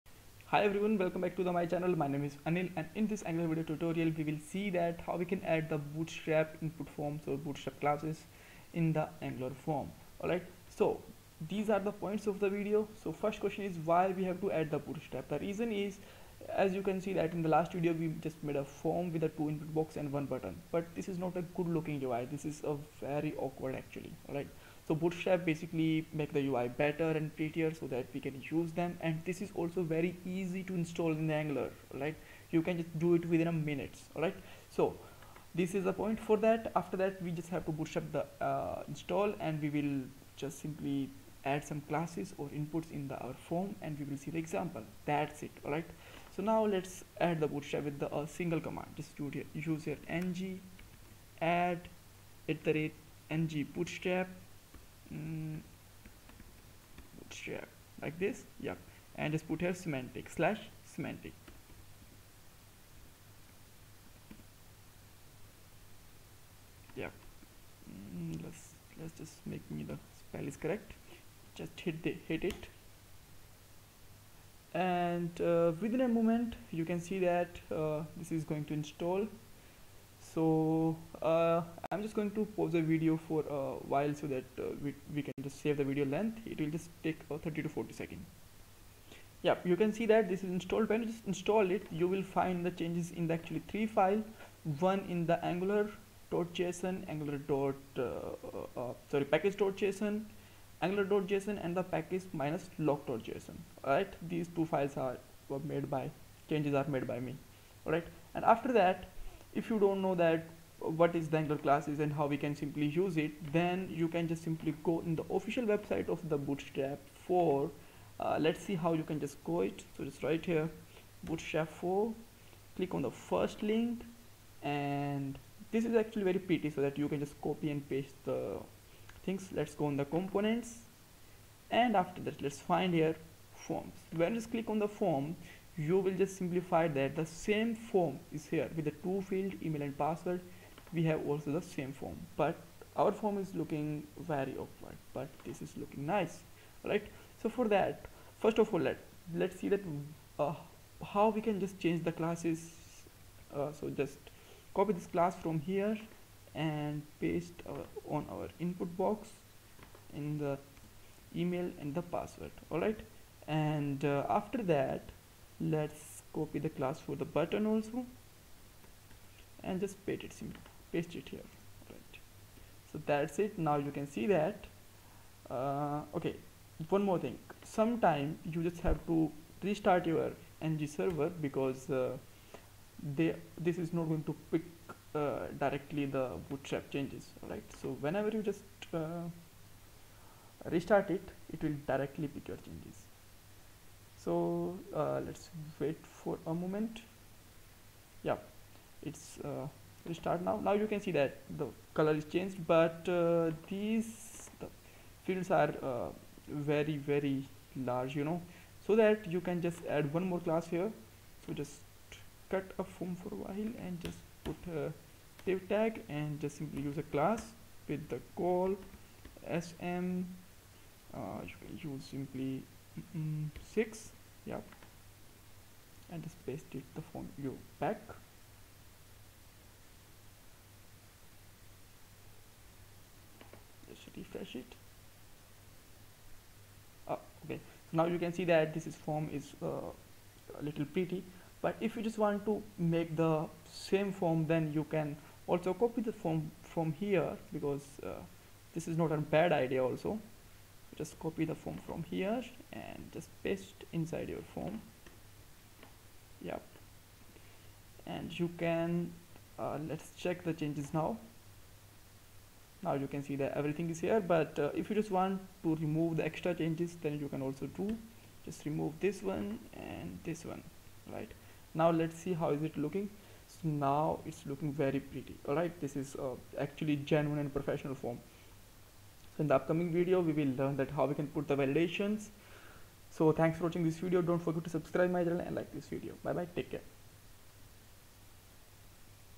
Angular forms tutorial # form with bootstrap
In this angular 9 tutorial, we will learn how er can add bootstrap class or input in the angular form with a simple example
steps of videos are here
Add Bootstrap
install Bootstrap
add Bootstrap input or class
example